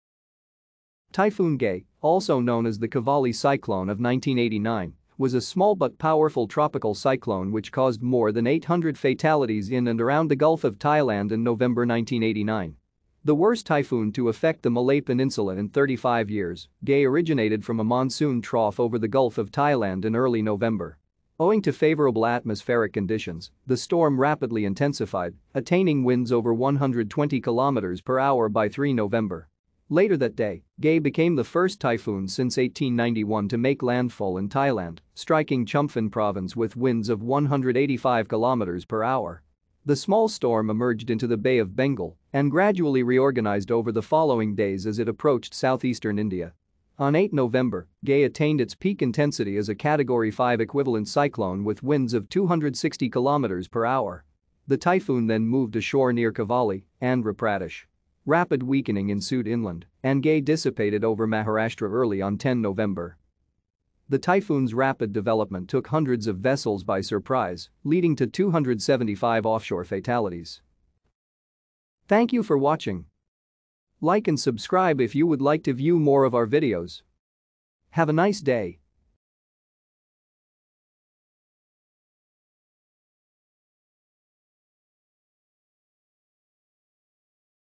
Typhoon Gay (1989)
Typhoon Gay, also known as the Kavali Cyclone of 1989, was a small but powerful tropical cyclone which caused more than 800 fatalities in and around the Gulf of Thailand in November 1989. The worst typhoon to affect the Malay Peninsula in thirty-five years, Gay originated from a monsoon trough over the Gulf of Thailand in early November. Owing to favorable atmospheric conditions, the storm rapidly intensified, attaining winds over 120 km/h  by 3 November. Later that day, Gay became the first typhoon since 1891 to make landfall in Thailand, striking Chumphon Province with winds of 185 km/h. The small storm emerged into the Bay of Bengal and gradually reorganized over the following days as it approached southeastern India. On 8 November, Gay attained its peak intensity as a Category 5-equivalent cyclone with winds of 260 km/h. The typhoon then moved ashore near Kavali, Andhra Pradesh. Rapid weakening ensued inland, and Gay dissipated over Maharashtra early on 10 November.